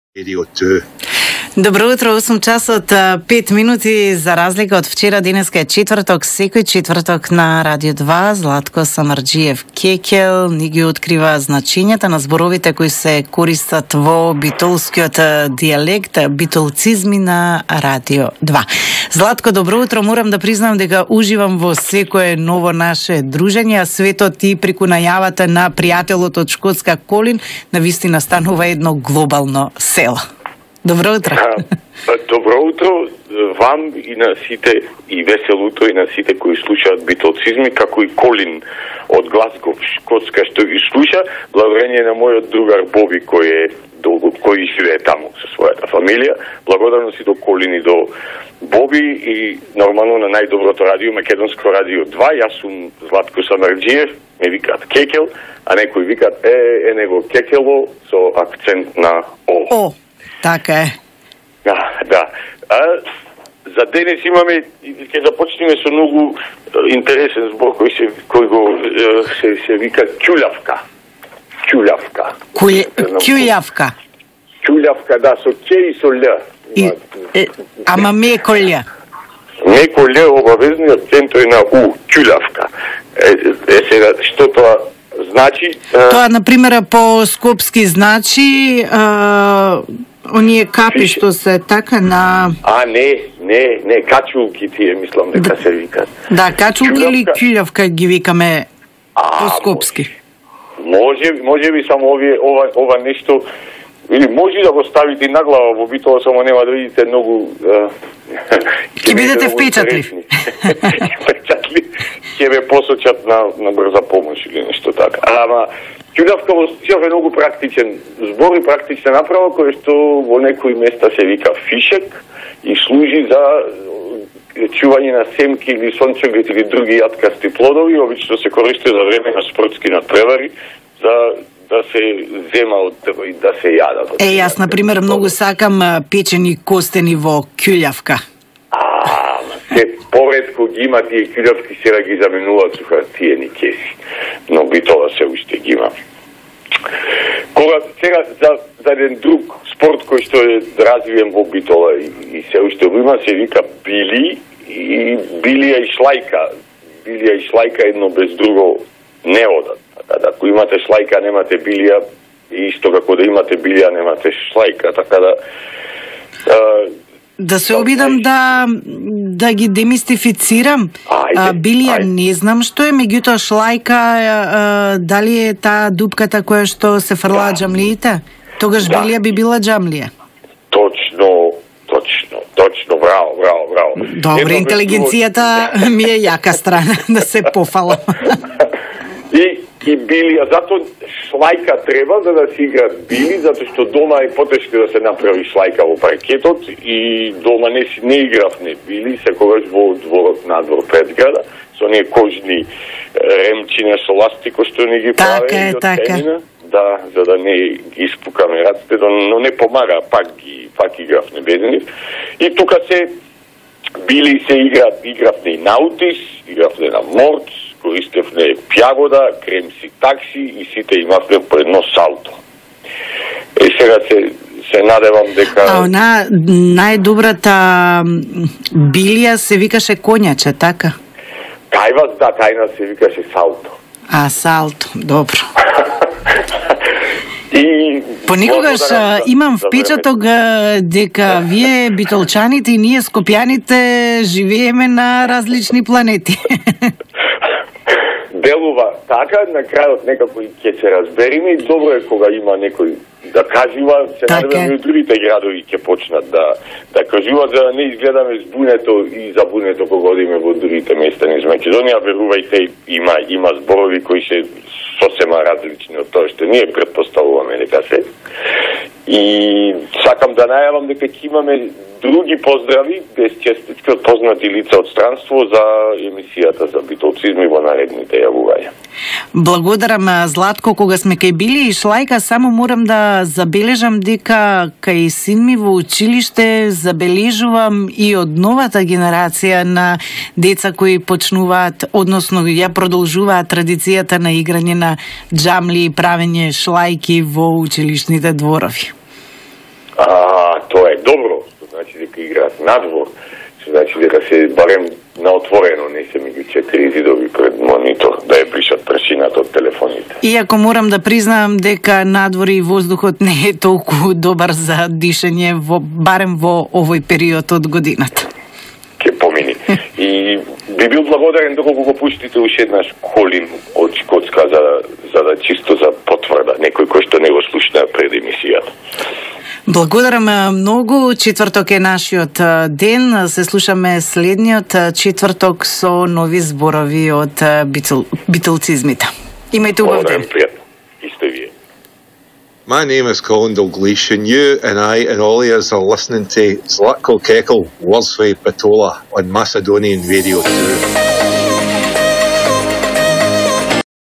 Bitolcuzmi kjuljafka bilii shlajka 17112016 so Scotland
Битолцизми ќуљафка, билии, шлајка со најава на Колин од Шкотска